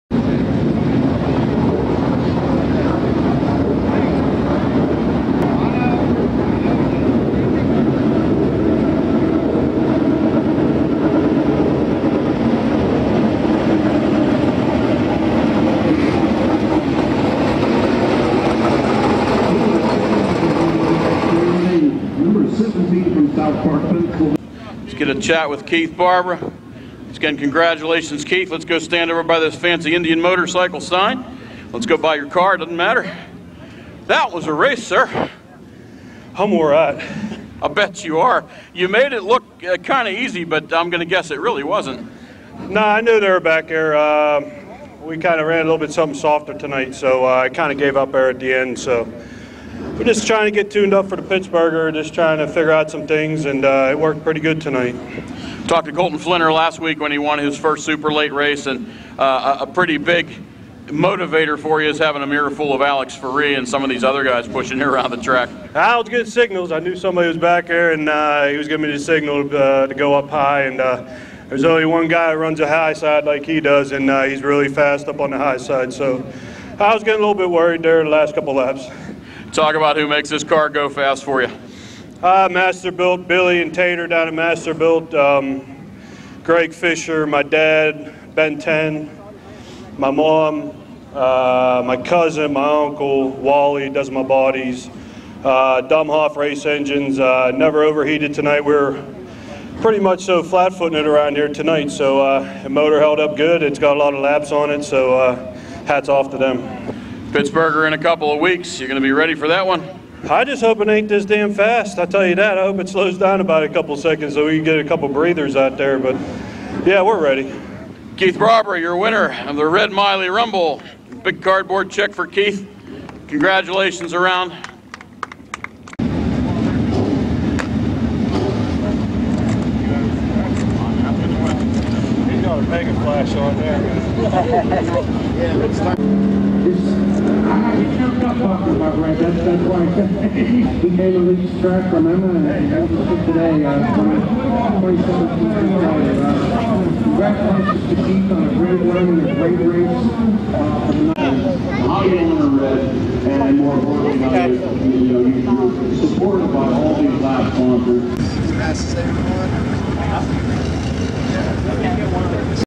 RED MILEY RUMBLE winner Barbara held on for the win with Ferree literally on his bumper
Davey Johnson and Keith Barbara started on the front row for the RED MILEY MEMORIAL. Johnson jumped into the lead at the drop of the green and led until lap 17, when Keith Barbara made a pass for the lead. Barbara was sailing along with a 3 second advantage until the only caution waved on lap 27. Alex Ferree took over second place, and running the high groove kept closing the gap on Barbara, got a great run out of the 4th turn on the final lap, but Barbara held on for the win with Ferree literally on his bumper.